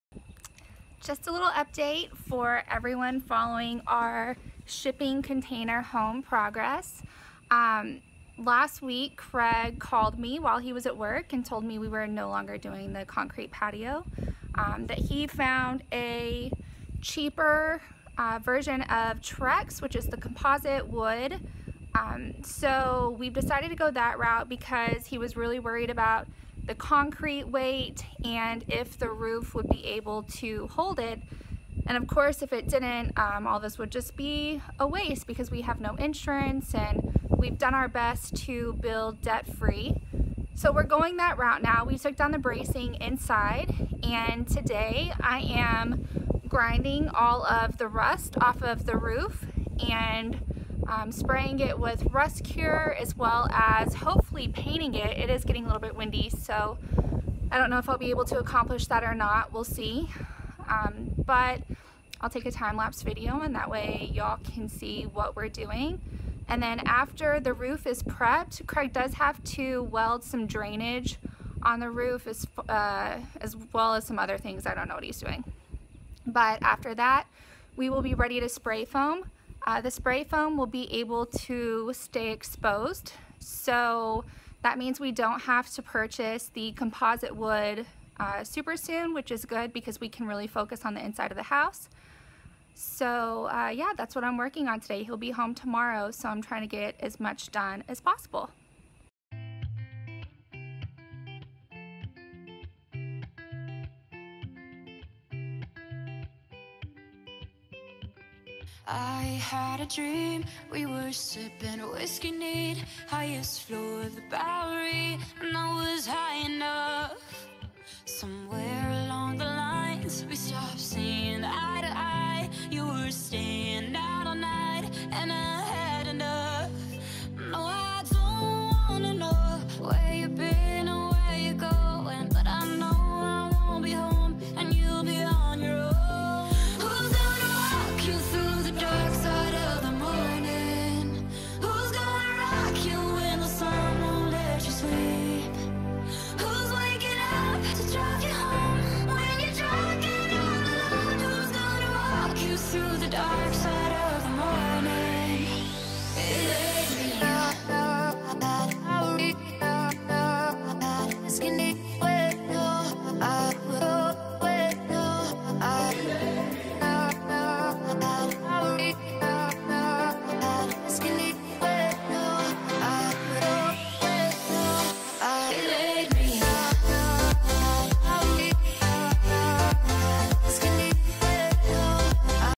Shipping Container Home Build - Rooftop Patio Update & Prep
Update on our rooftop patio!  We've decided against the concrete patio because of the risk and are going with composite wood decking.  Right now, we are prepping for the spray foam to be sprayed.